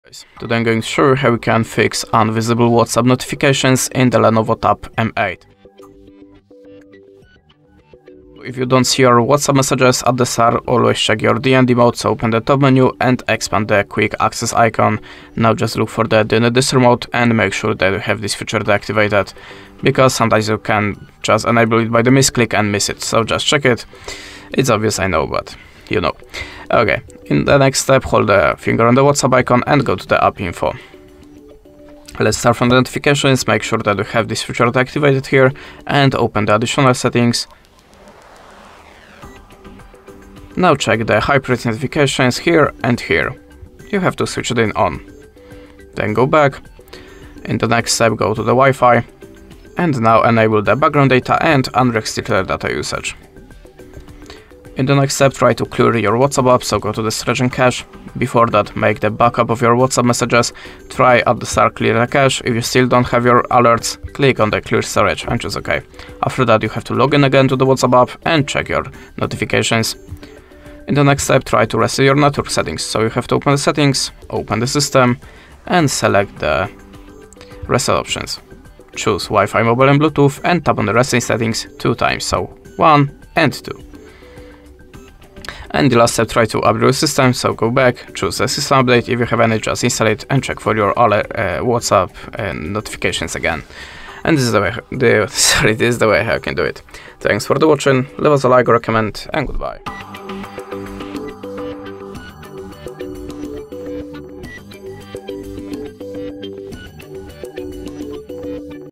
How to Fix Invisible Whatsapp Notifications on LENOVO Tab M8?
Find out more info about LENOVO Tab M8
You will be guided through troubleshooting steps in this tutorial to resolve the issue of WhatsApp notifications not appearing properly on the LENOVO Tab M8. You can experience seamless notification visibility on WhatsApp by resolving the problem of invisible notifications. Learn how you can make WhatsApp notifications appear clearly and promptly on your LENOVO Tab M8.

What causes WhatsApp notifications to become invisible on the LENOVO Tab M8?
Where can I find the settings to manage WhatsApp notifications on this device?
How do invisible notifications impact the usage of WhatsApp on the LENOVO Tab M8?
Can I adjust the settings to make WhatsApp notifications visible on the device?
Does the tutorial cover additional troubleshooting steps beyond settings adjustments?